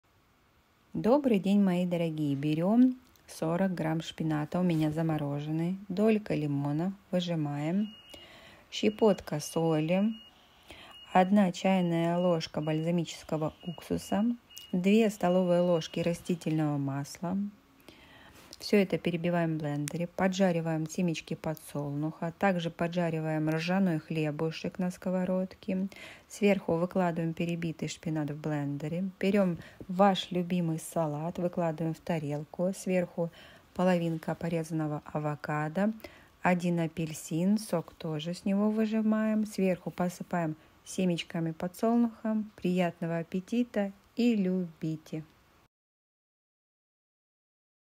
Брускетта со шпинатом и салат с апельсином
40 г шпината
10 г лимона, долька
1 ч  ложка бальзамического уксуса
Щепотка соли
2 с л  растительного масла
1 шт ржаного хлеба
1 ст л семена подсолнуха
Горсть салата
1 шт апельсин
1/2 шт авокадо

Приятного аппетита и любите💐💕